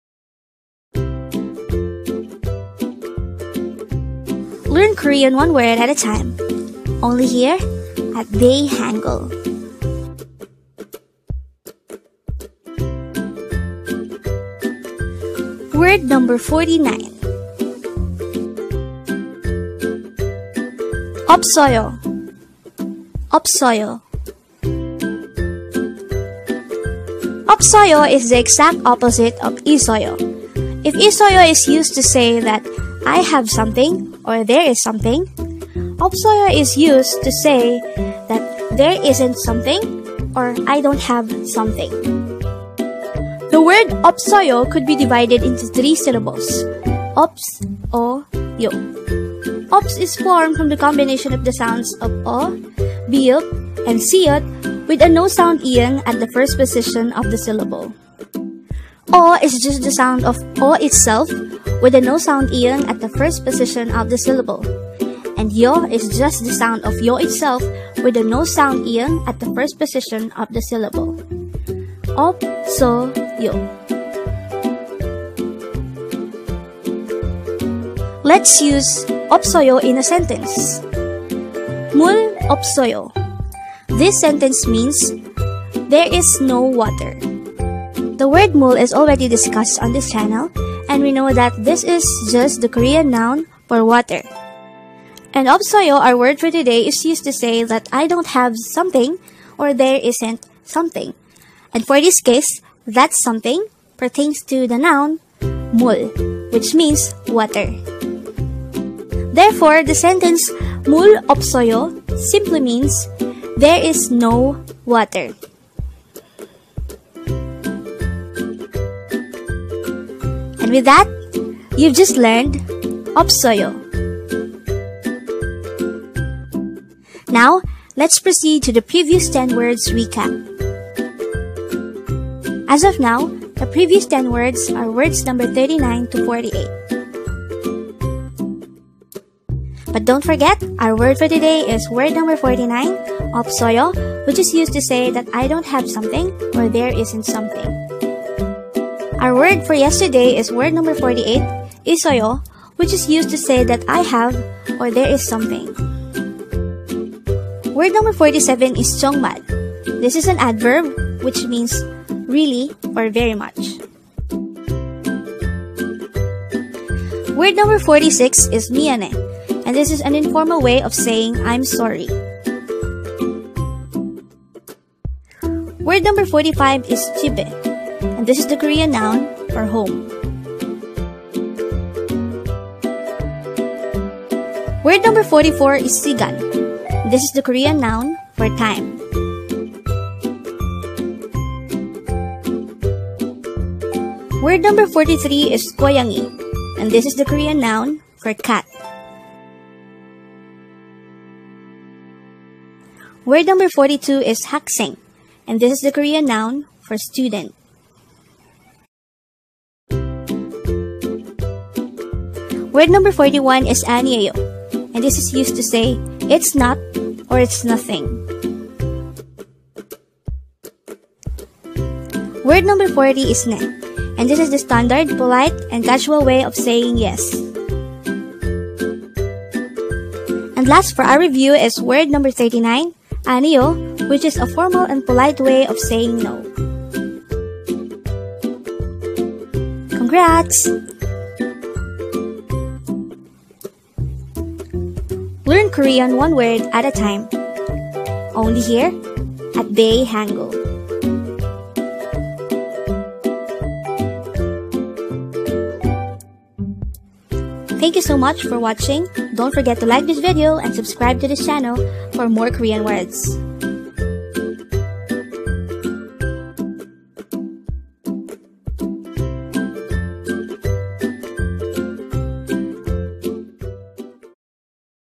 EOPSEOYO | WORD NO. 49 | 1 KOREAN WORD A DAY | DAE-HANGUL
TURN ON CAPTIONS FOR ENGLISH SUBTITLES!

Learning korean language doesn't have to be overwhelming but has to be a discipline. Learn korean by memorizing at least one word a day, and if we can, let's learn more in a day!

Here's our dose of korean for today!
Word No. 49: EOPSEOYO (없어요)